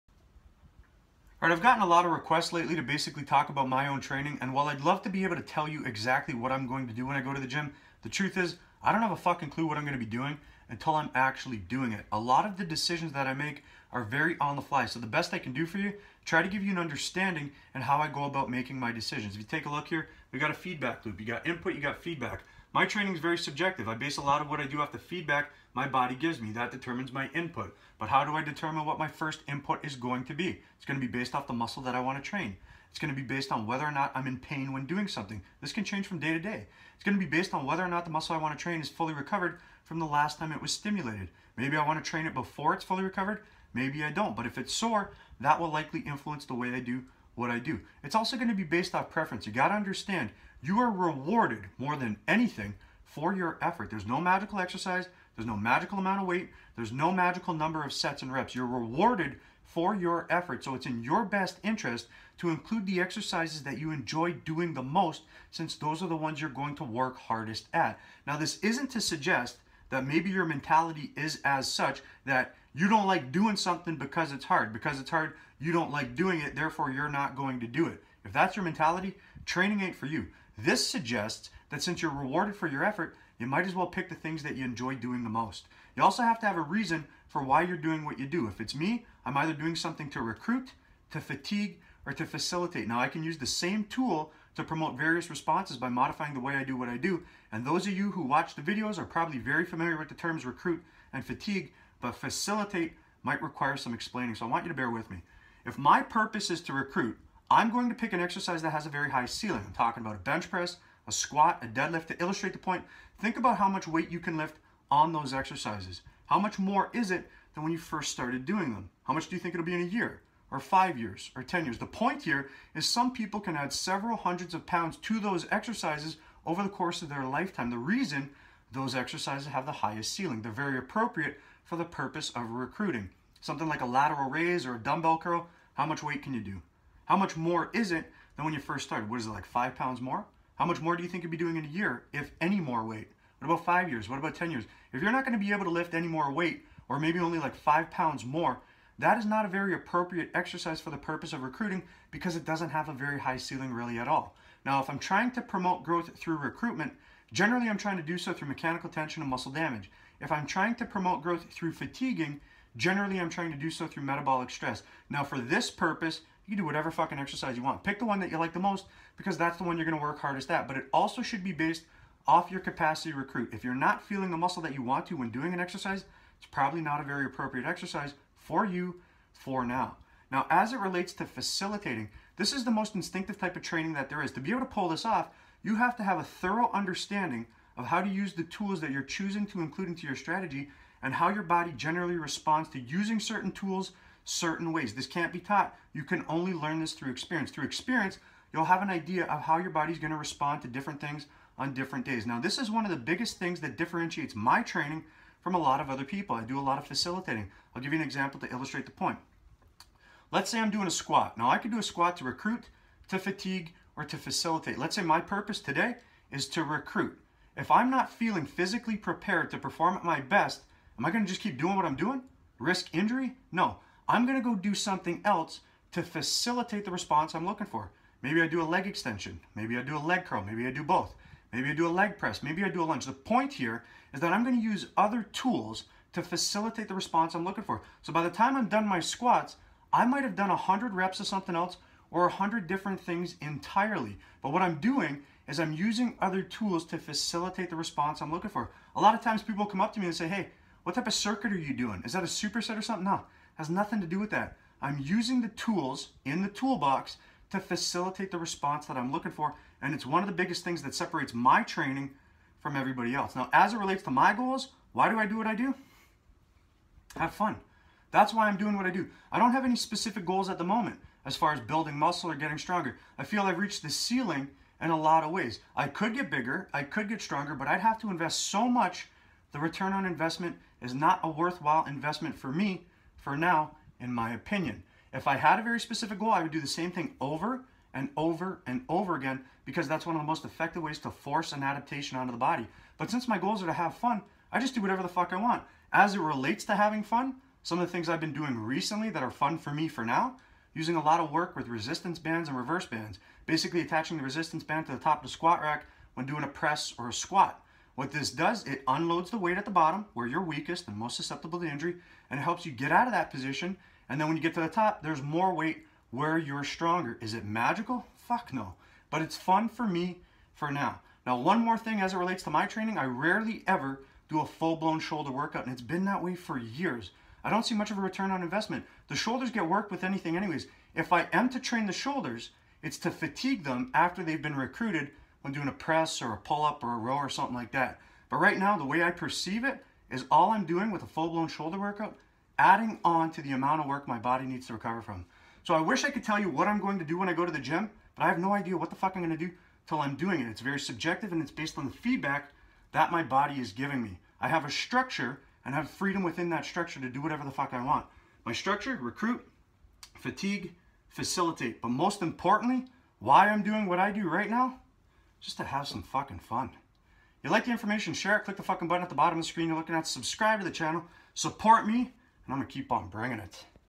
What My Training Currently Looks Like (inspired by those who have asked "what do I do?")
I've had some requests to share what my own training is like, so I figured I'd try to present/illustrate my mindset when making decisions as it relates to my training. Because my primary goal (which is subject to change) is to "have fun," it's impossible for me to forecast just exactly what I'm going to do before I'm actually doing it.

My own training is very subjective, and I base what I do next, off how my body is responding to what I've just done. What I first choose to do is based off a lot of different things, and from there, I make decisions on the fly. I have a structure that I work within, but I have freedom within that structure to do whatever I feel like.

If there is one consistency with my training, it's that I never really train shoulders - not for a lack of desire, as they are among my favorite body parts to train, but rather the return on investment is low. They receive a ton of stimulation from a lot of other exercises I include in my strategy, so to include a full blown shoulder workout would be increasing the amount of work my body needs to recover from, without much of a return on investment.